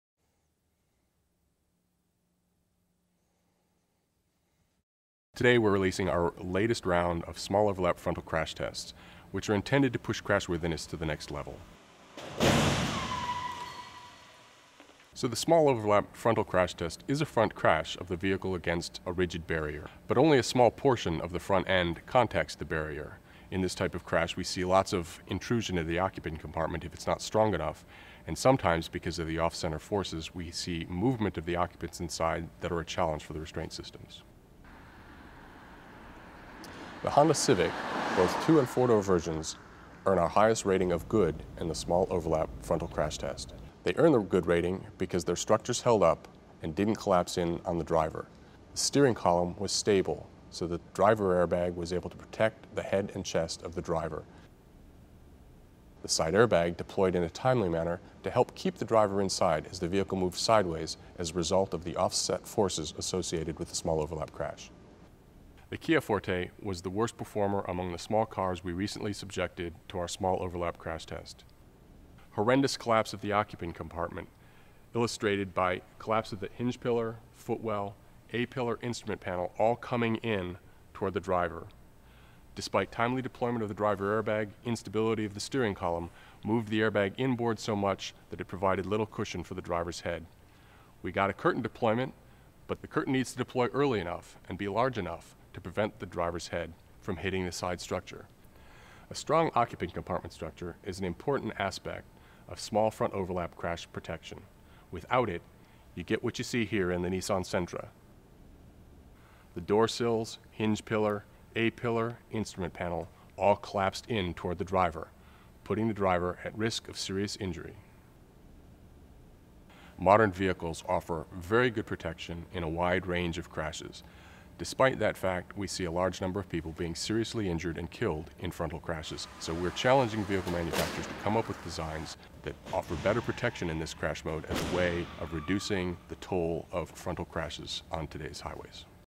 IIHS News: Small overlap crash test results for small cars
ARLINGTON, Va. — The latest small overlap front crash test results from the Insurance Institute for Highway Safety (IIHS) reveal a range of performance among many of the best-selling small cars in the U.S. market. Of the 12 models evaluated, half earn a good or acceptable rating and qualify for the IIHS Top Safety Pick+ award.